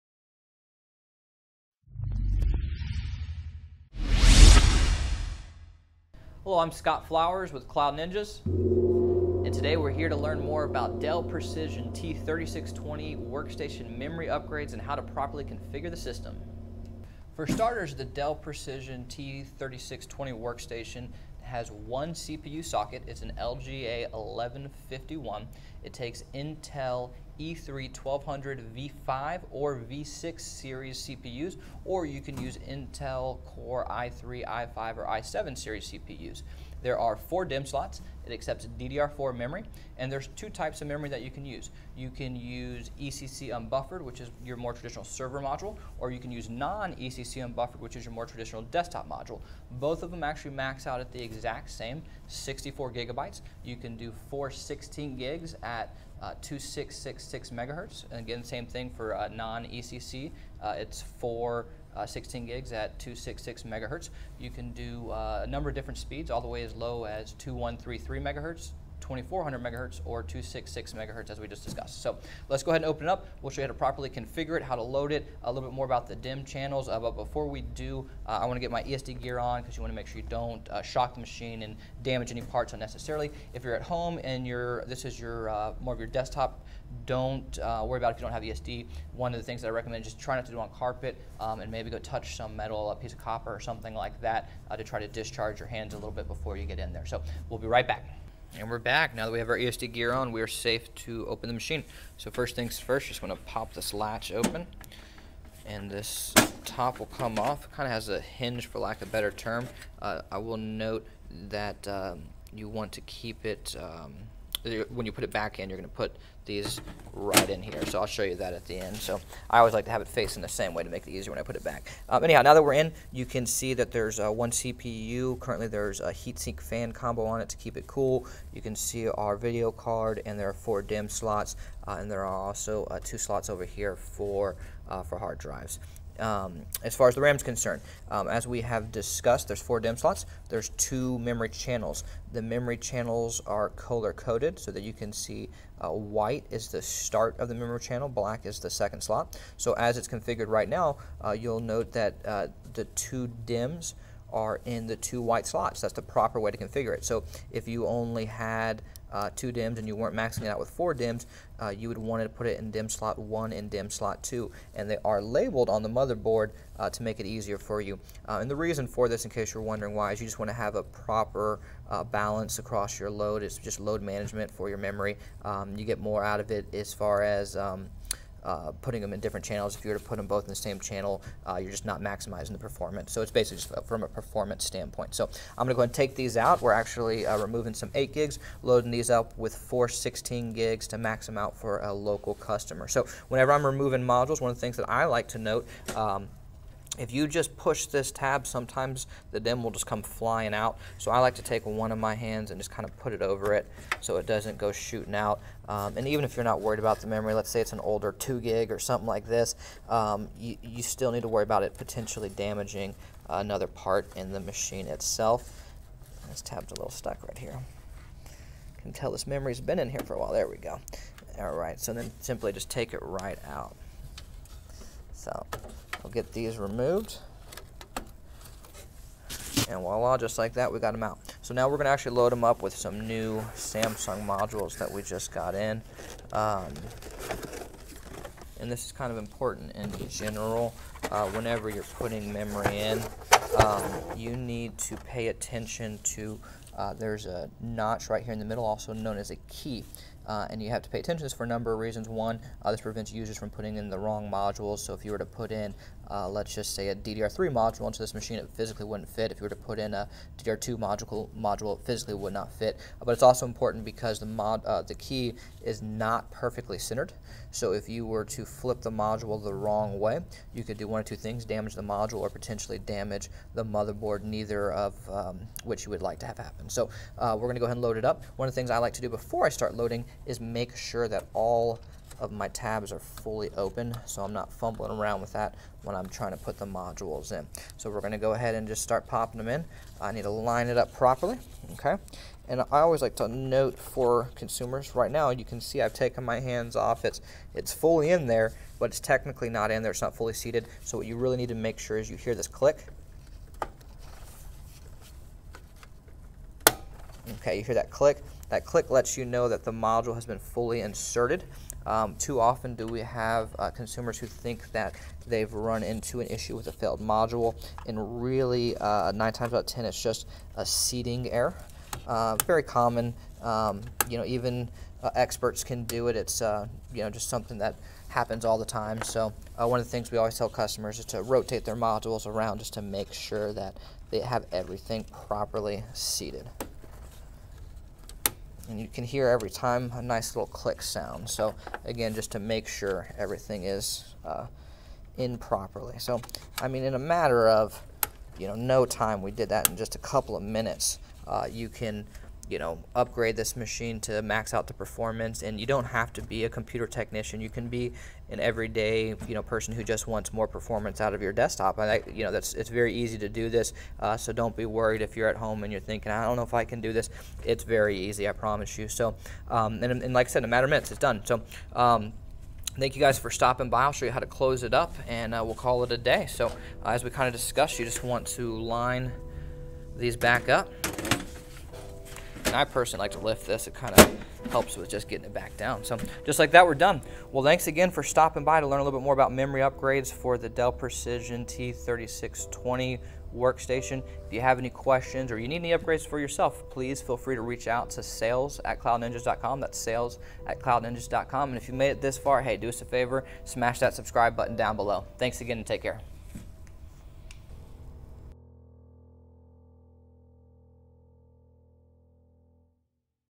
Dell Precision T3620 Tower Workstation | Memory Upgrade Options | How To Install & Configure RAM
Dell Precision T3620 | Learn the different types of memory accepted by the Dell 3620 workstation and exactly how to upgrade the memory inside.  In our video, we will review the proper techniques to install your memory as well as a helpful overview on correct way to configure your RAM.  We will provide a general overview of the system to help familiarize yourself with the T3620 as a whole.

For starters, the Dell Precision T3620 workstation can be used for a ton of different applications from intense gaming to everyday desktop internet browsing.  This is an amazing choice for a fast, reliable system.  There is 1 CPU socket which is an LGA1151.  The 3620 workstation most notably utilizes Intel E3-1200 V5 or V6 series CPUs.  You also have the choice of Intel Core i3, i5 or i7 series processors.  The Dell T3620 desktop tower has 4 DIMMs rocking out DDR4 memory.  Speed options are 2133MHz, 2400MHz or 2666MHz.  DIMM Capacity sizes include 4GB, 8GB or 16GBs.  There are two types of compatible memory:  ECC Unbuffered, also know as a Server UDIMM, or Non-ECC Unbuffered which is your standard desktop memory module.  Both are good options, even though we recommend the ECC UDIMMs over the Non-ECC UDIMMs.  ECC = Error Correction Code and is a nice feature to have.  The max configuration is the same for both types of RAM.  You can officially max out at 64GB via 4 x 16GB DDR4 2666MHz ECC Unbuffered Server Memory.  Unfortunately 32GB DIMMs are not accepted.  We did try.  They do not registered so 4 x 16GB = 64GB is in fact the max.  Also it is good to note - ECC Registered (RDIMMs) or ECC Load Reduced (LRDIMMs) modules will not work with the Dell Precision T3620 workstation. We will cover all this and more in our video.